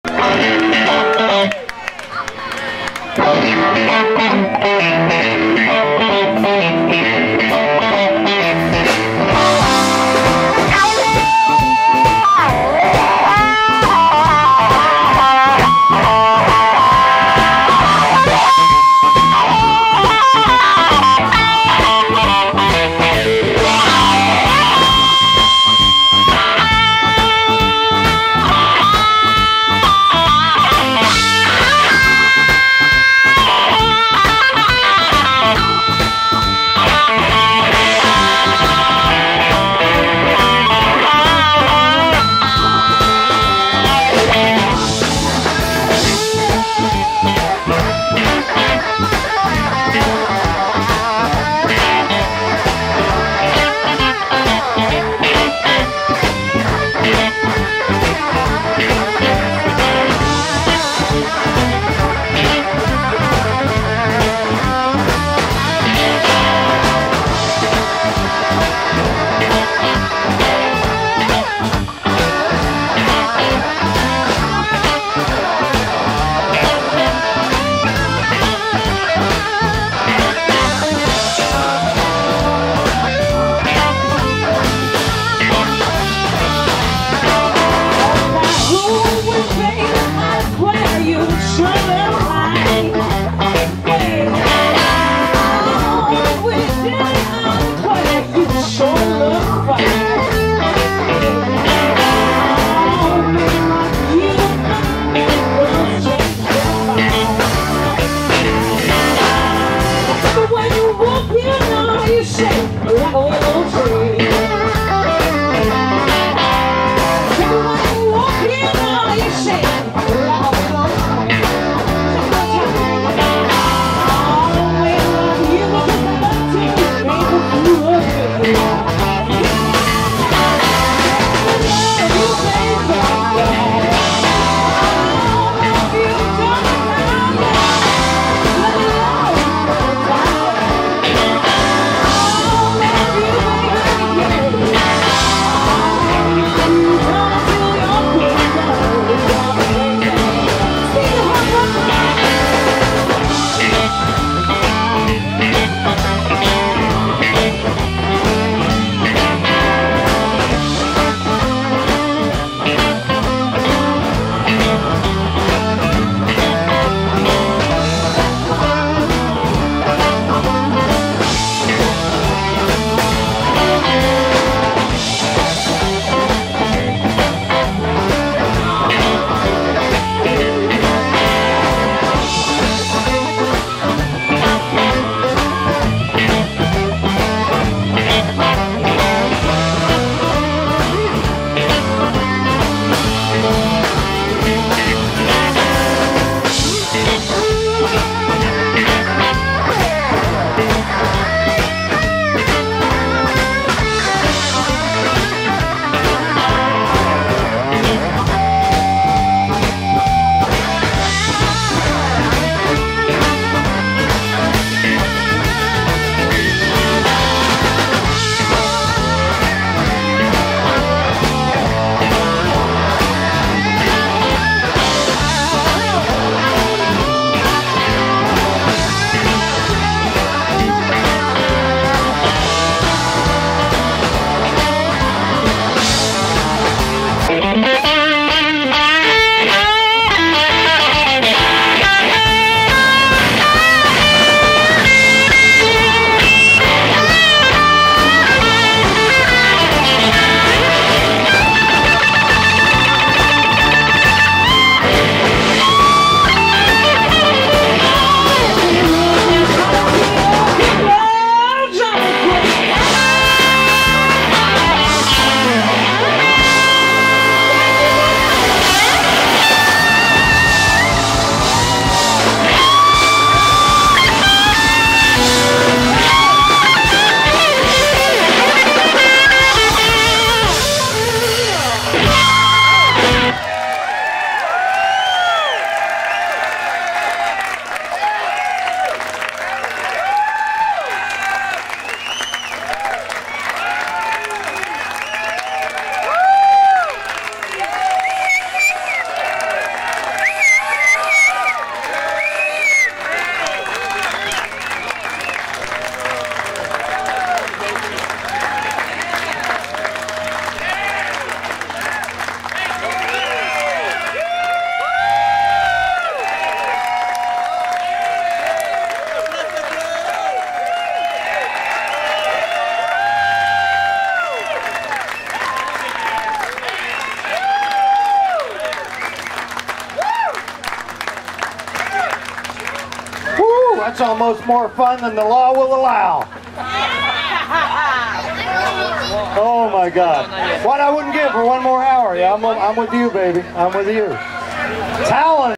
Ana Popovic, Tallan Noble Latz and Steve"The Harp"
This video is Tallan Noble Latz and Steve "The Harp" sitting in with the Ana Popovic Band! from the 2012 Ain't Nothing but the Blues Festival.  It was Ana's encore and it was set up very last minute as the video shows. They had to squeeze this in as time was running out, all music had to stop before a certain time.  They were in a rush to finish.